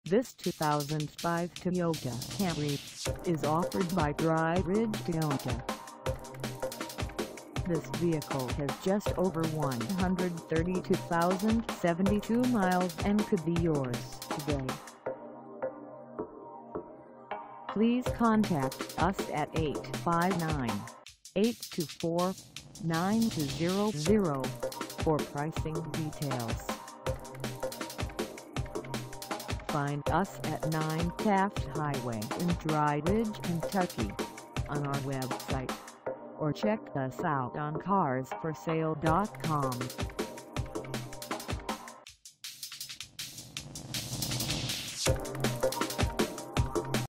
2005 Toyota Camry for sale in Dry Ridge, KY 41035 at Dry Ri
This 2005 Toyota Camry  is for sale in Dry Ridge, KY 41035 at Dry Ridge Toyota.
Engine: 4-Cylinder 2.4L I4 SMPI DOHC
Transmission: 5-Speed
Color: Maroon
Mileage: 132072
Contact Dry Ridge Toyota 9 Taft Highway Dry Ridge, KY 41035 to test drive this 2005 Toyota Camry  today.